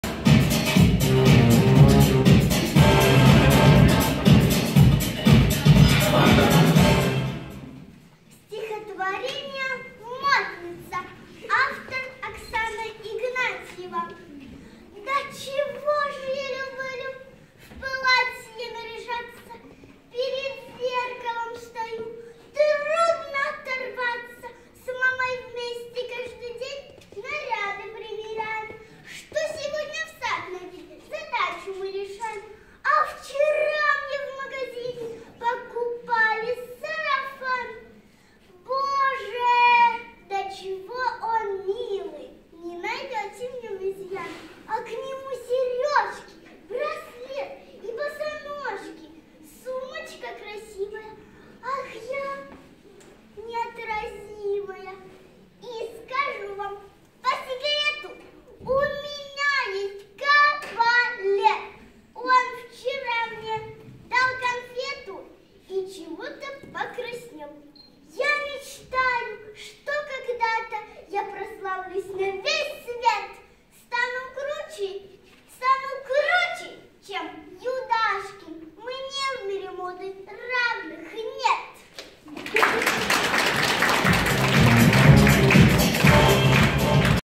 Рауан 2017 Виктория Соломахина Стих "Модница" д/с №2
Первый отборочный тур на фестиваль детского творчества Рауан 2017 г. Павлодар д/с №85
Авторы постановок - педагоги дошкольных учреждений г.Павлодара. Спасибо большое авторам!